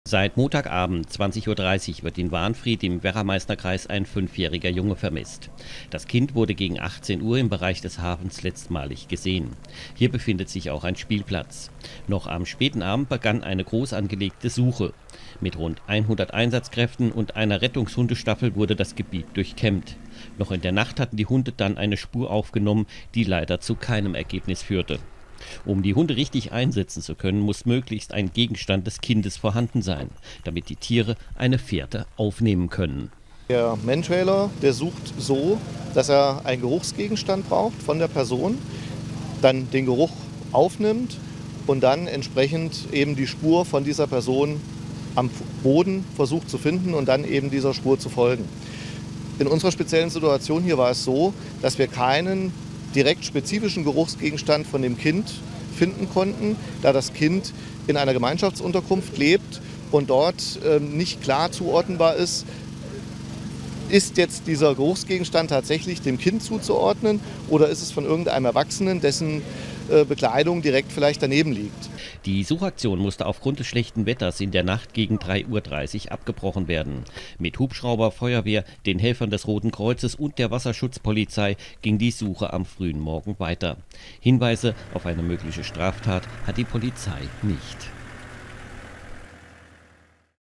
Wanfried: Fünfjähriger vermisst - Polizei sucht mit Großaufgebot
Wanfried, 5. April 2016. Ein Fünfjähriger wird seit Montagabend in Wanfried vermisst. Gegen 18 Uhr verschwand der Junge, zuletzt wurde er im Bereich des Wanfrieder Hafens gesehen. Am Morgen wurde die Suche nach ihm fortgesetzt.